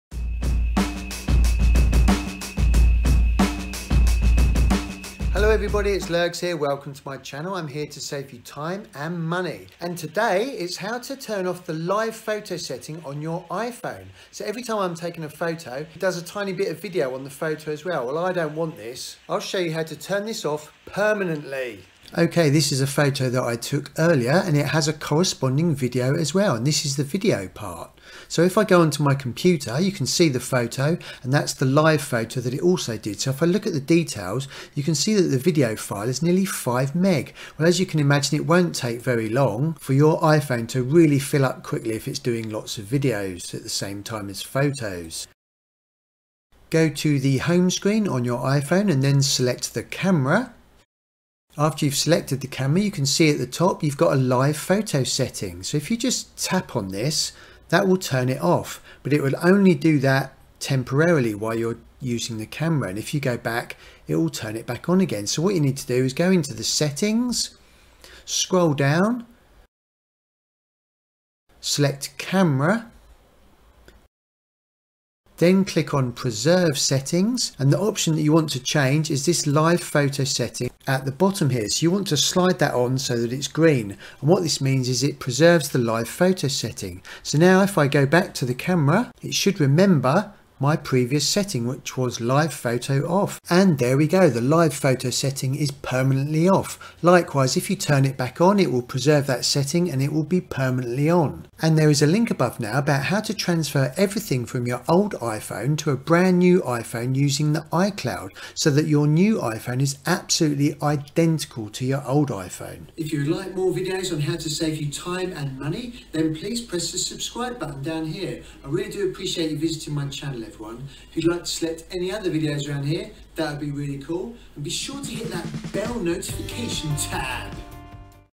How to turn off Live Photos on iPhone permanently | iPhone Disable Live Photos permanently
How to turn off Live Photos on iPhone permanently.
iPhone Disable Live Photos.
If you have iPhone Live Photo setting on it will soon fill up your iPhone storage as every photo you take it will create an iPhone Live Video.
In this video I will show you how to permanently DISABLE the iPhone Live Photos setting.